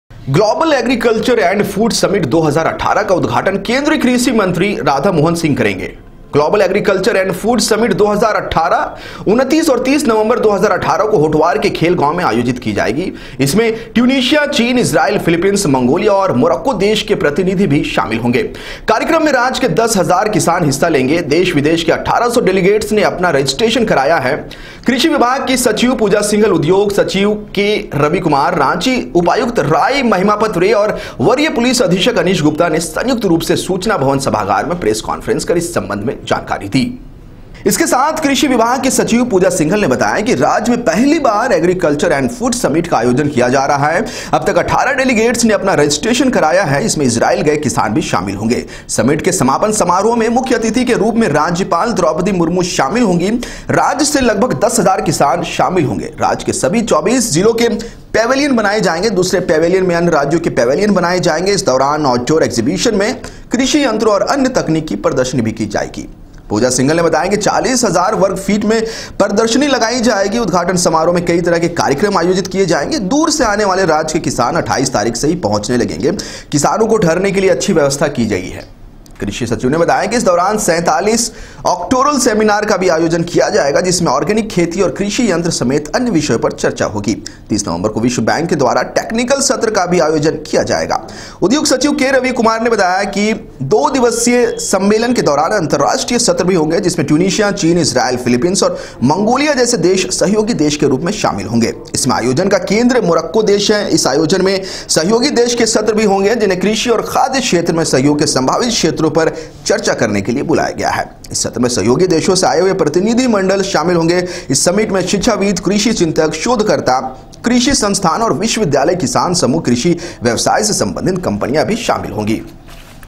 रांची। ग्लोबल एग्रीकल्चर समिट की तैयारी पूरी
रांची। रांची के होटवार में होनेवाले ग्लोबल एग्रीकल्चर समिट की तैयारी पूरी कर ली गई है। एंक रिपोर्ट...